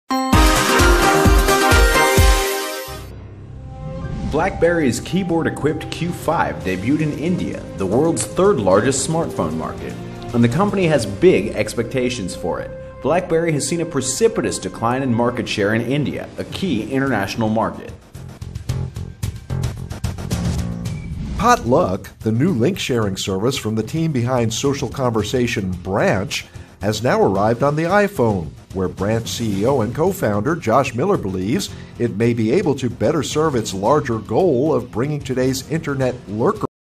MOBILE Breaking News BlackBerry Targets India
BlackBerry?s keyboard-equipped Q5 debuted in India, the world?s third-largest smartphone market, and the company has big expectations for it. BlackBerry has seen a precipitous decline in market share in India, a key international market Potluck, the new link-sharing service from the team behind social conversation Branch, has now arrived on the iPhone, where Branch CEO and co-founder Josh Miller believes it may be able to better serve its larger goal of bringing today's Internet "lurkers" out of the shadows. KeepSafe, which creates a private photo vault on people's phones, just raised $3.4 million in a round led by Floodgate. They've also grown to 13 million users and 6 million of those are active every month. The app creates a password-locked photo gallery on people's phones.